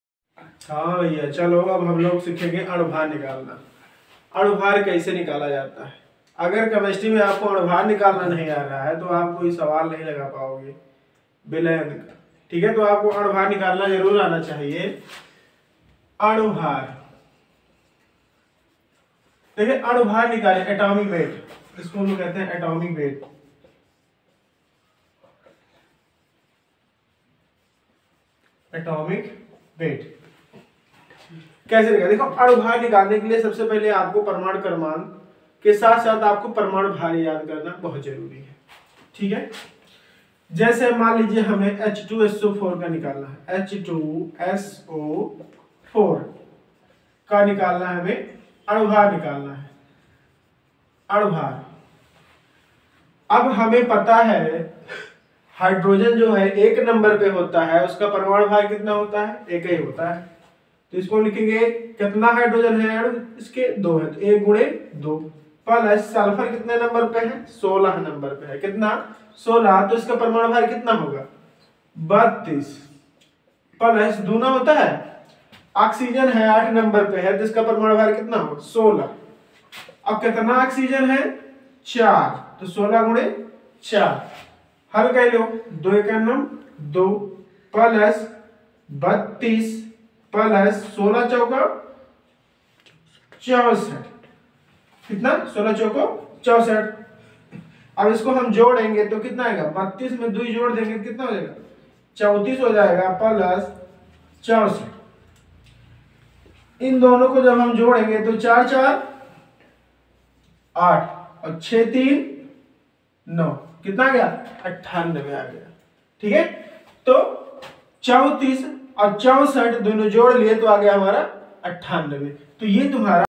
अणु भार कैसे निकालते हैं (Atomic weight) l anubhar kaise nikaalte hain l
अणु भार कैसे निकालते हैं (Atomic weight) l anubhar kaise nikaalte hain l#anubhar kaise nikaalte hain
atomic weight
anubhaar ka prayog
use of atomic weight
The molecular mass is the mass of a given molecule: it is measured in daltons. Different molecules of the same compound may have different molecular masses because they contain The molecular weight (relative molecular mass) represents an important parameter in molecular characterization. It is related to many other molecular properties, as shown in Chapter 4. Also, a common purpose of MW in HPLC is its use in the differentiation between small molecules and macromolecules.The molecular weight of a compound is the sum of the atomic weights of the atoms in the molecules that form these compounds. Example: The molecular weight of the sugar molecule found in cane sugar is the sum of the atomic weights of the 12 carbon atoms, 22 hydrogen atoms, and 11 oxygen atoms in a C12H22O11 molecule.